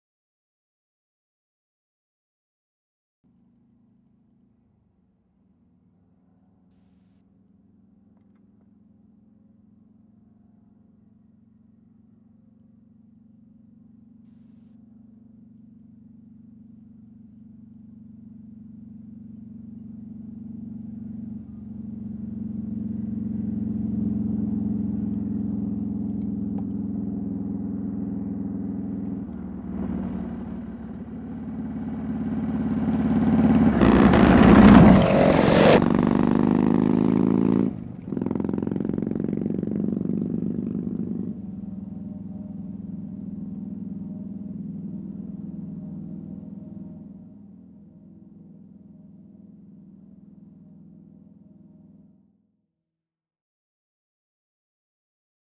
elvis at bonneville speed week 09
elvis blew by us while we were watching the long coarse runs!